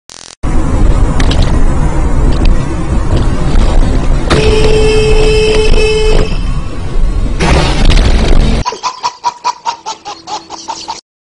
Scusami se ho suonato il clacson!
Mai suonare il clacson!  potresti confondere le idee già confuse del conducente che si sta confondendo guidando confusamente una macchina!